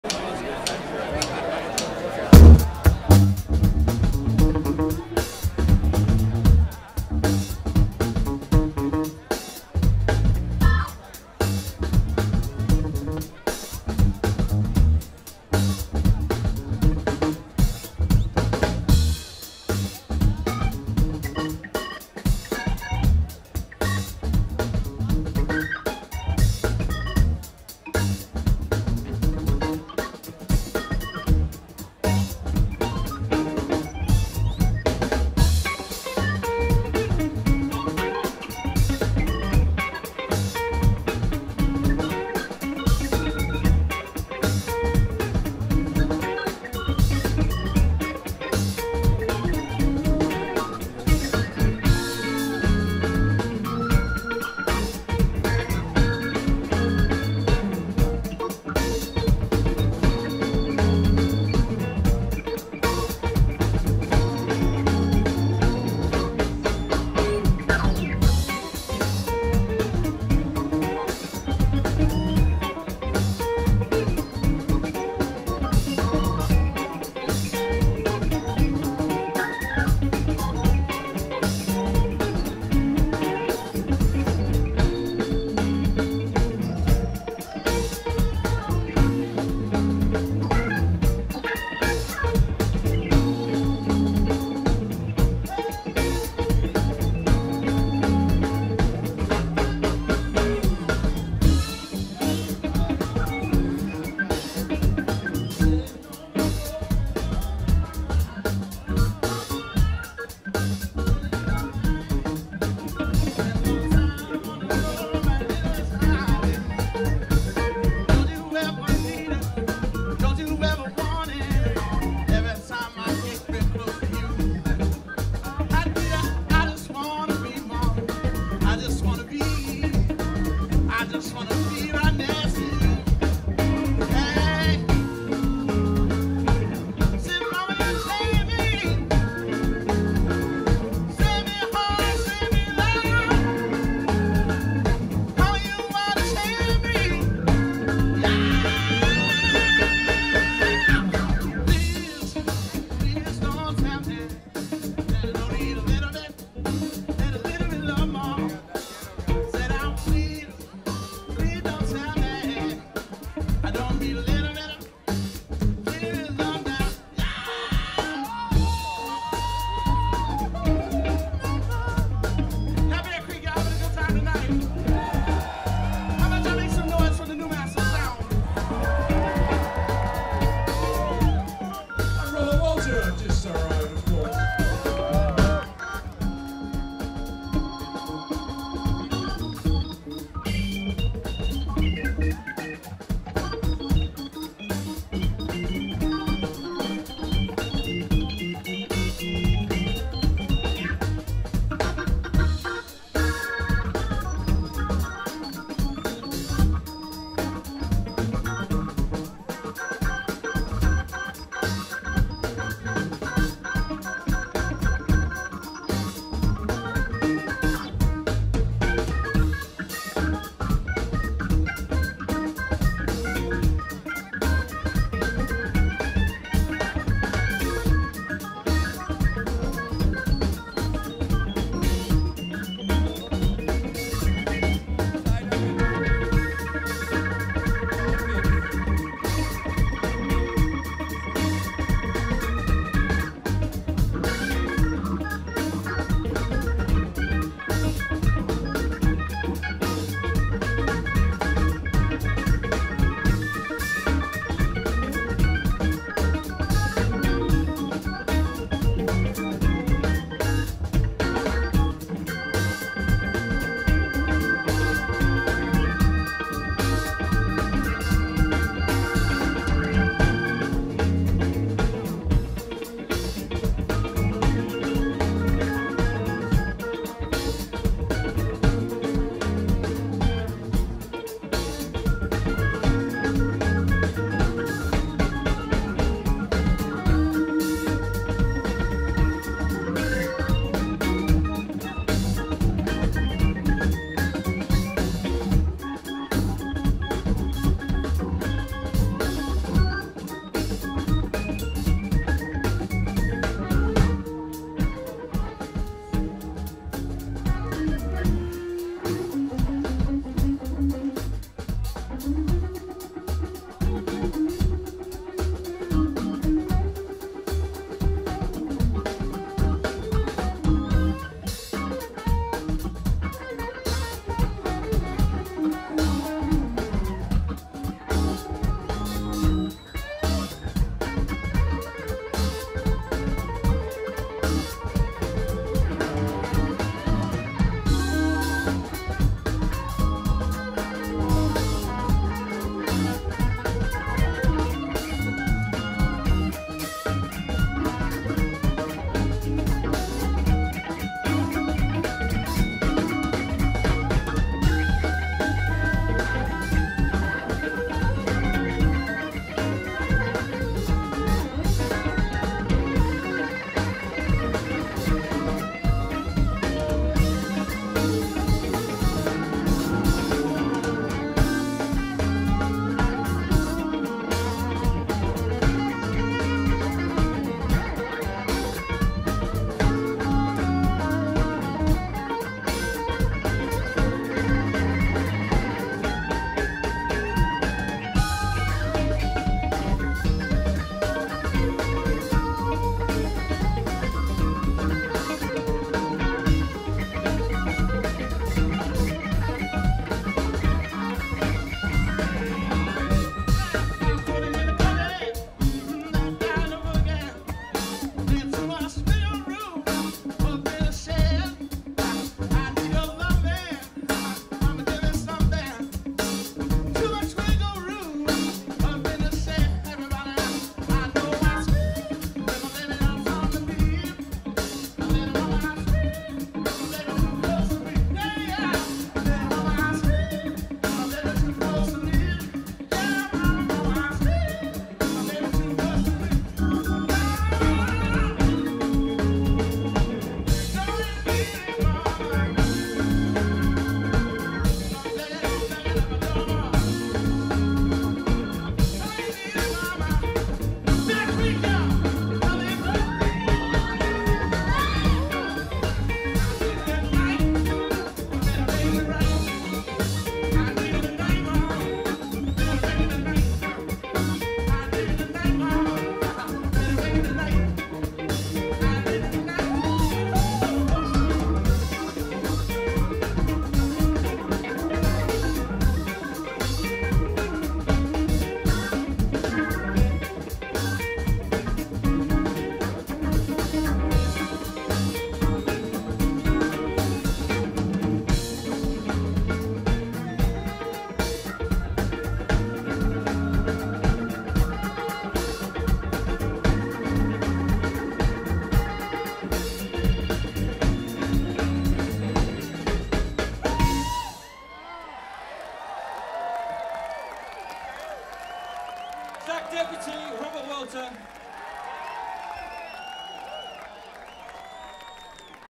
The New Mastersounds Live @ Bear Creek Music Festival 11-12-2011
The New Mastersounds with Zach Deputy & Robert Walter Live @ Bear Creek Music Festival, Uncle Charlie's Porch Stage, Live Oak, FL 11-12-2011. Recorded by adam @ CHeeSeHeaDPRoDuCTioNS.